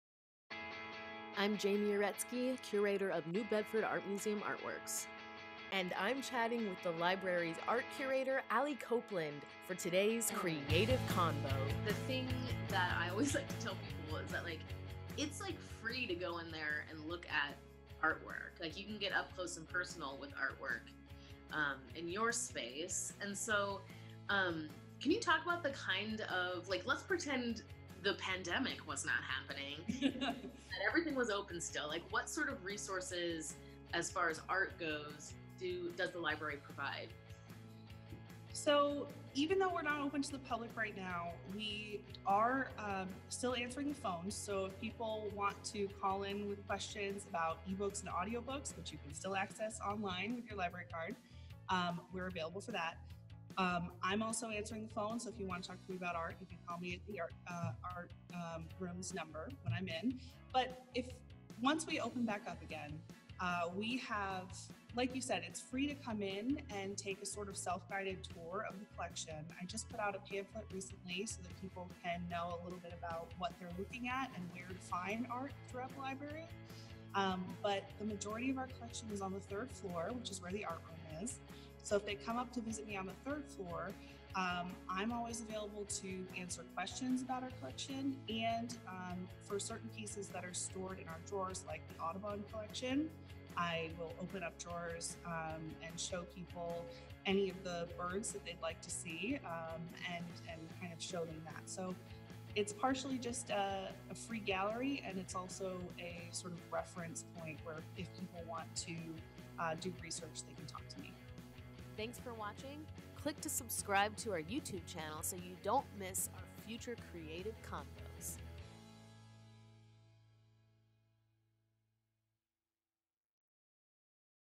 Creative Convos! Episode 4: Curator Allie Copeland on NBFPL
NBAM's Curator Jamie Uretsky chats with New Bedford Free Public Library's Curator Allie Copeland about the Library's art collection.

Since 1852, the library has amassed extensive holdings of printed works, manuscripts, graphics, fine art, and objects used for research, exhibit, and educational purposes. The art collection is on public view at the Main Library, and special exhibitions, curated by Allie Copeland, are showcased in the City Gallery at the New Bedford Art Museum.